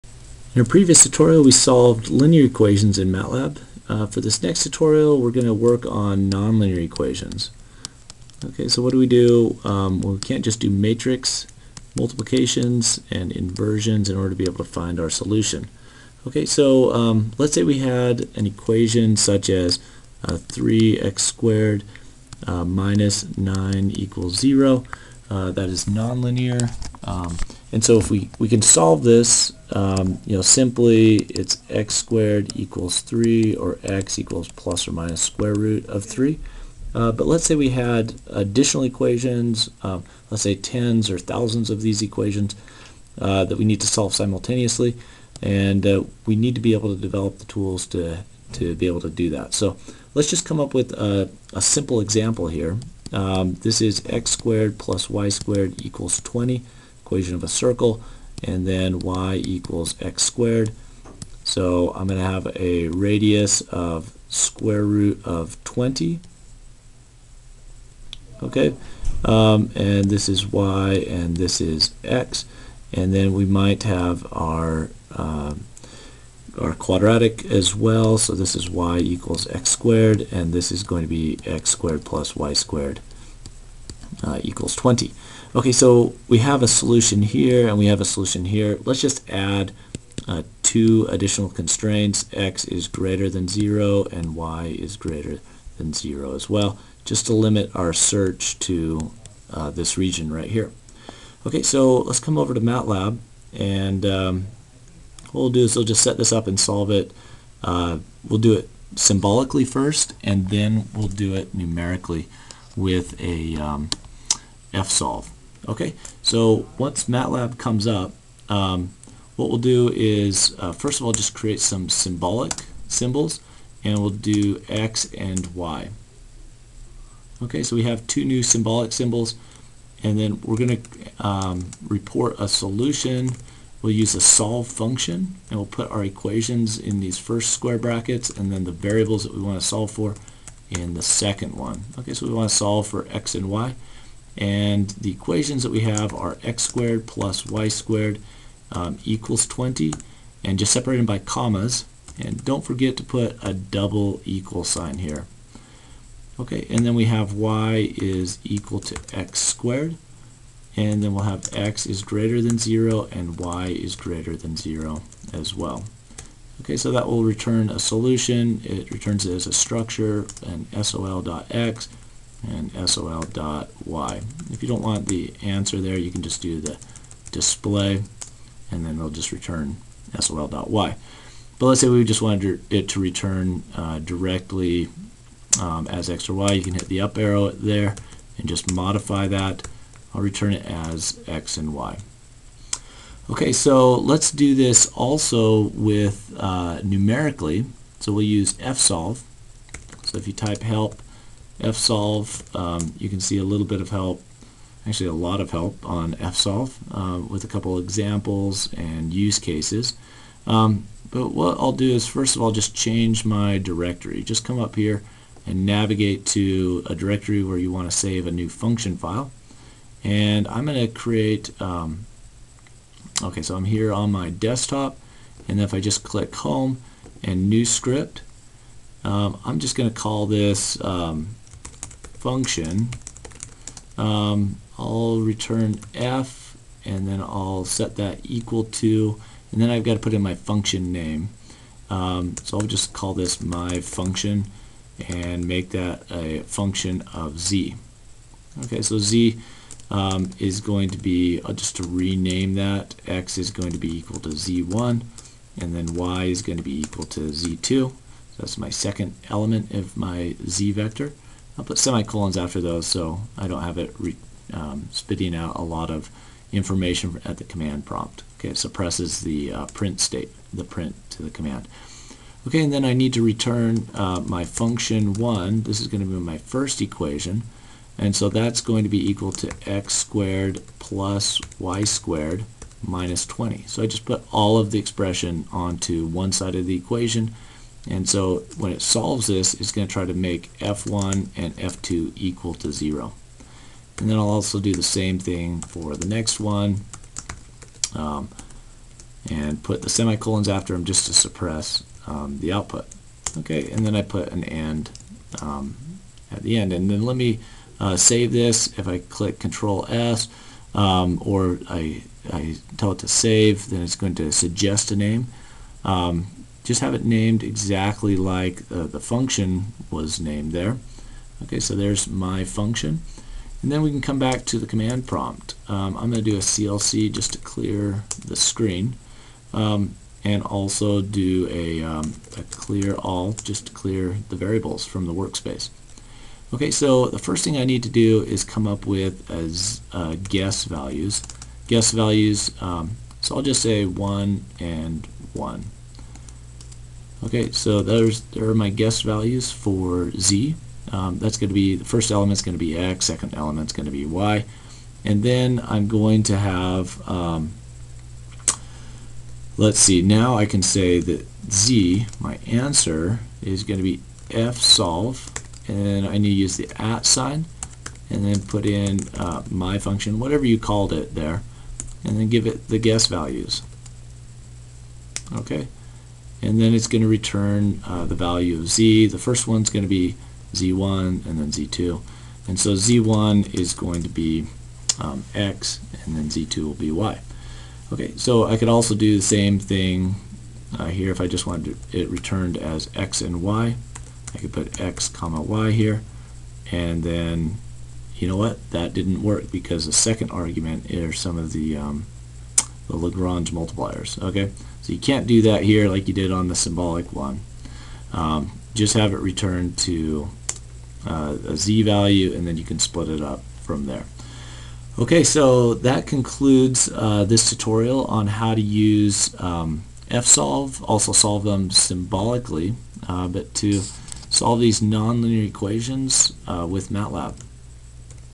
Solve Nonlinear Equations with MATLAB
MATLAB solves nonlinear equations either symbolically or numerically with solvers such as "fsolve". This example demonstrates how to obtain a solution to sets of nonlinear equations.